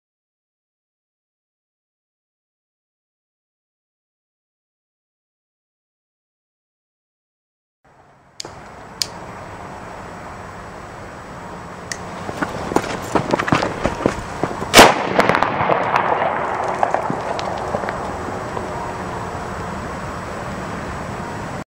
Selfmade Cracker (in 3D)
Kleiner Cracker. Mann hört leider auf dem Video den Schall nicht so ;) die explosion ging eigentlich. Die Zündschnur war halt nur etwas kurz.